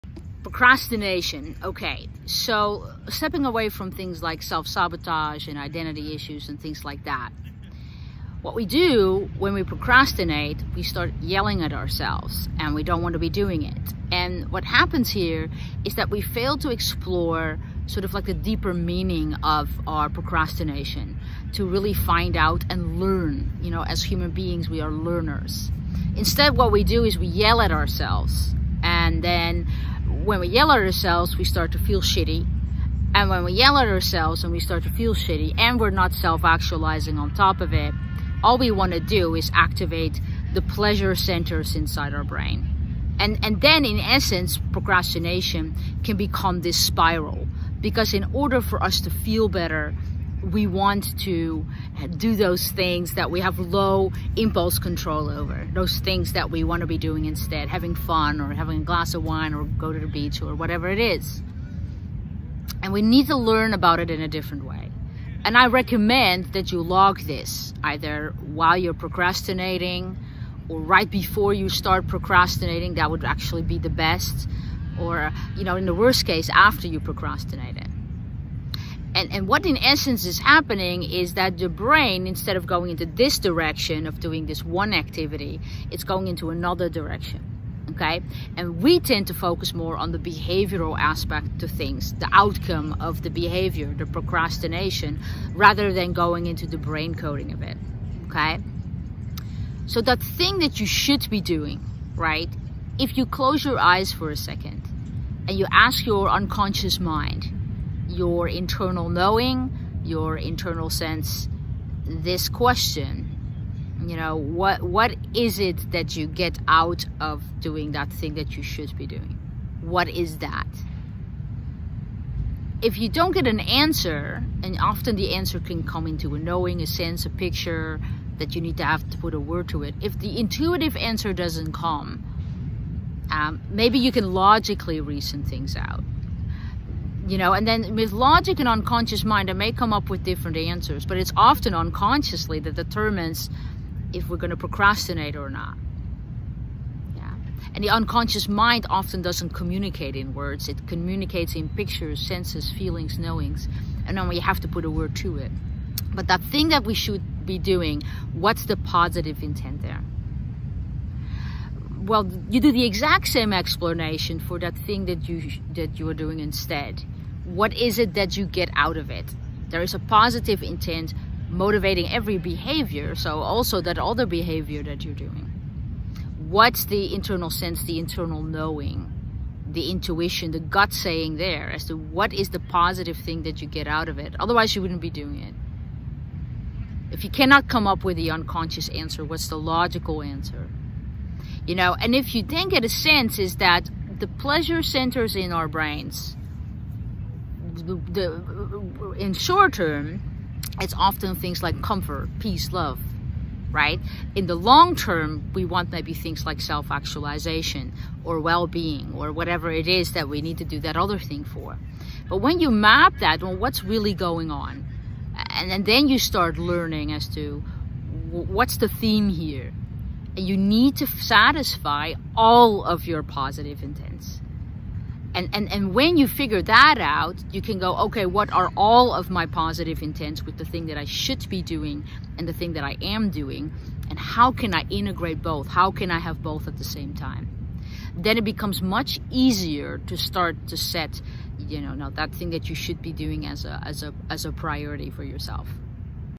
NLP Training: How to Fix Procrastination
How to fix procrastination was a question asked during the last NLP training in Los Angeles. In this video, I will give you an entirely different approach where it comes to what procrastination is and how to fix it.

Both in our live as well as online NLP training we teach the following presupposition:
There is a positive intent motivating every behavior.

Even our bad behavior, in this case, procrastination we do for a very good reason. That doesn't relate to being lazy, distracted, self-sabotage or self-esteem.

And the moment we realize are driven by good intent,  we stop being angry with ourselves. we start getting closer to the fix to procrastination.

If you want to have more information about coaching, live or online NLP training